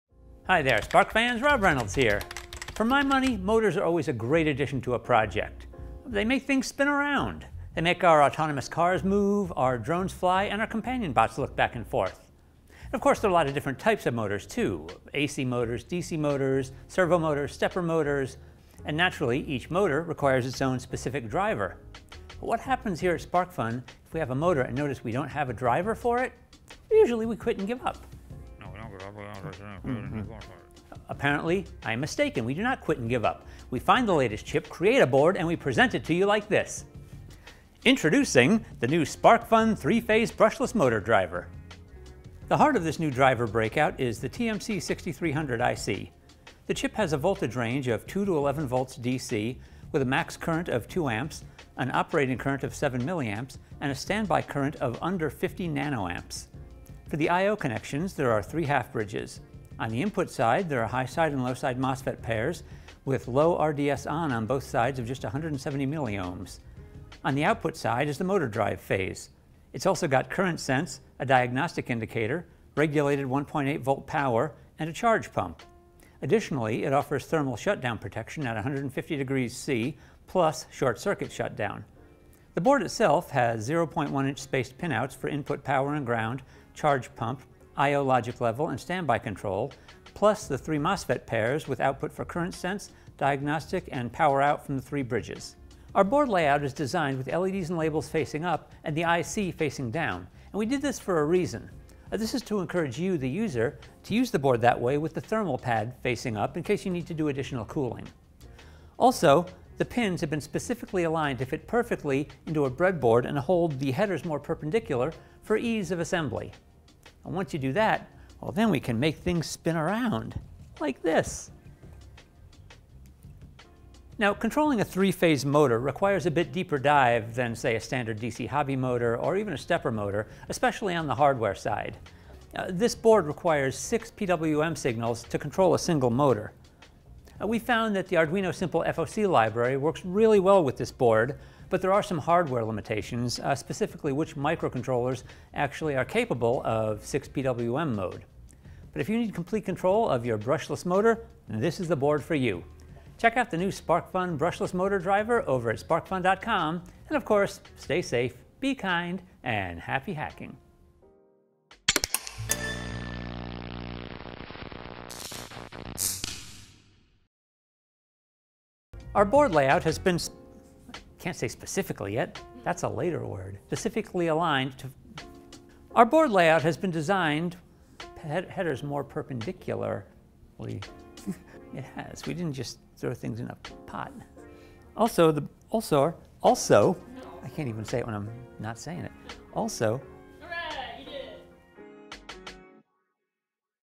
Product Showcase: SparkFun Brushless Motor Driver
The SparkFun 3-Phase Brushless Motor Driver, featuring the TMC6300 from ADI + Trinamic, is a powerful and easy-to-use three-phase motor driver that was designed to control our Brushless Gimbal Motor. However, it can be used to control any 3-phase BLDC or PMSM motor with up to 2A (1.4ARMS) of total drive current. Separate high-side and low-side control of the three half-bridges allows for incredible control of each phase of the motor commutation. The driver also provides temperature and short circuit protections, and a diagnostic output to indicate system faults. With a 1.8V regulated power output and an operating voltage down to 2V, the TMC6300 is suitable for low-power microcontroller and battery-powered designs (min. two AA/NiMh cells, or one to two Li-Ion cells).